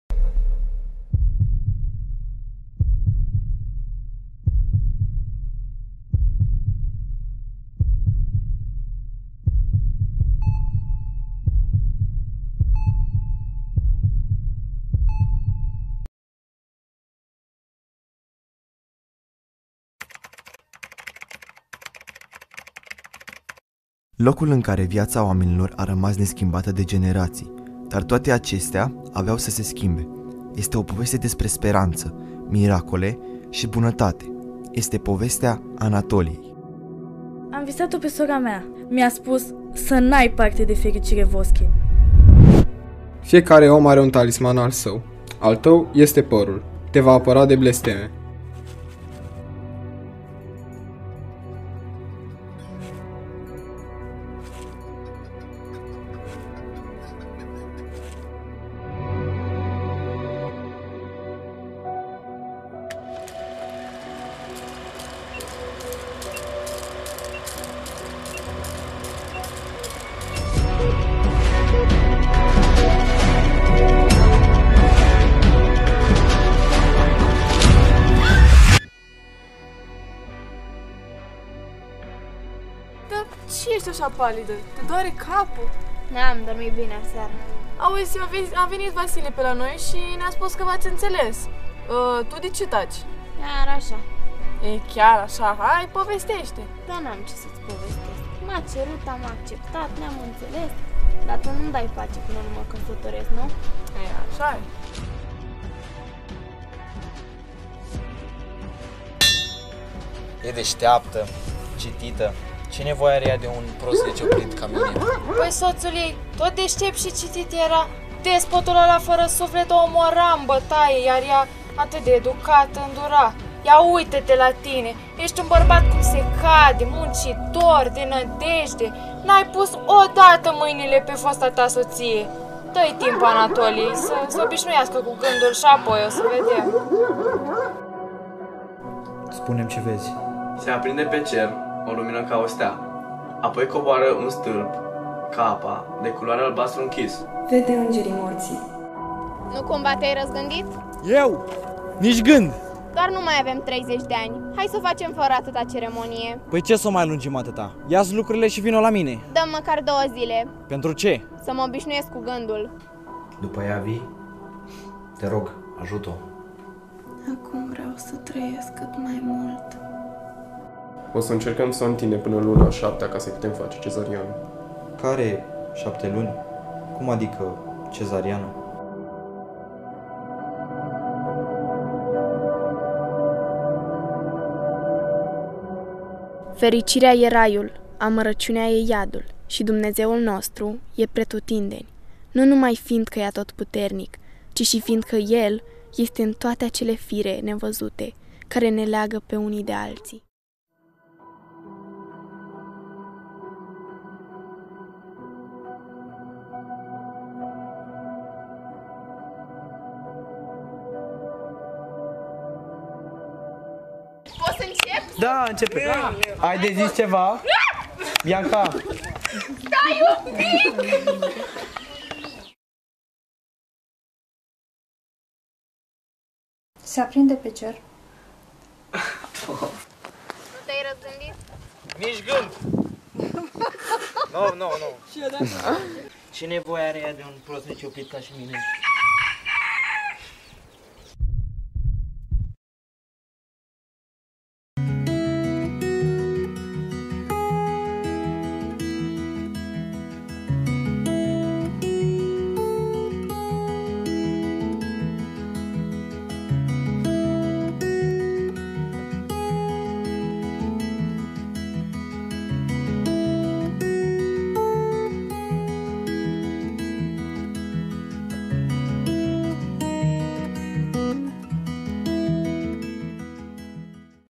4TEAM • Din cer au căzut trei mere • Boovie 2023
Trailerul echipei 4TEAM de la COLEGIUL NAŢIONAL "FERDINAND I" BACĂU, BACĂU pentru cartea Din cer au căzut trei mere de Narine Abgaryan
Team 4TEAM's trailer from COLEGIUL NAŢIONAL "FERDINAND I" BACĂU, BACĂU for Three apples fell from the sky by Narine Abgaryan